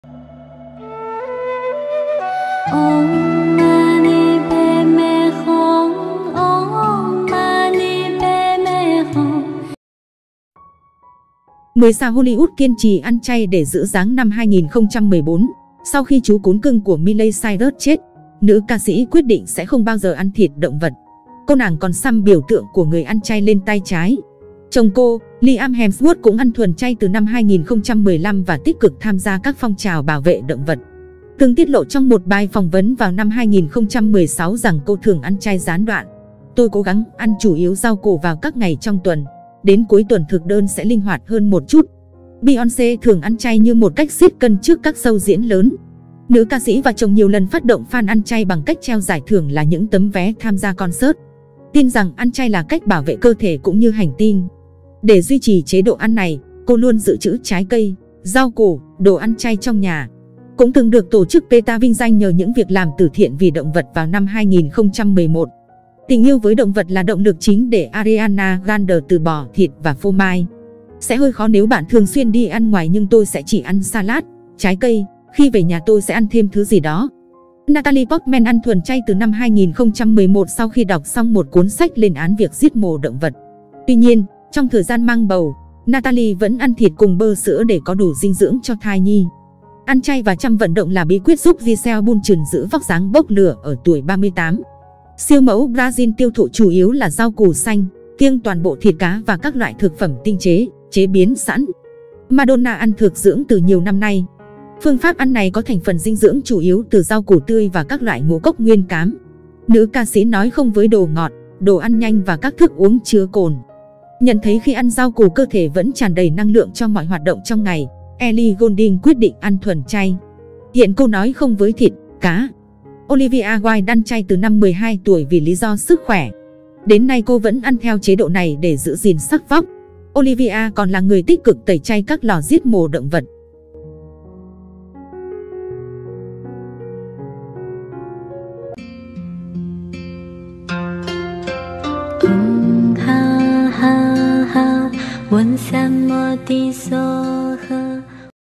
10 Ngôi Sao Hollywood thường duy trì chế độ ăn thuần chay
10 sao Hollywood kiên trì ăn chay để giữ dáng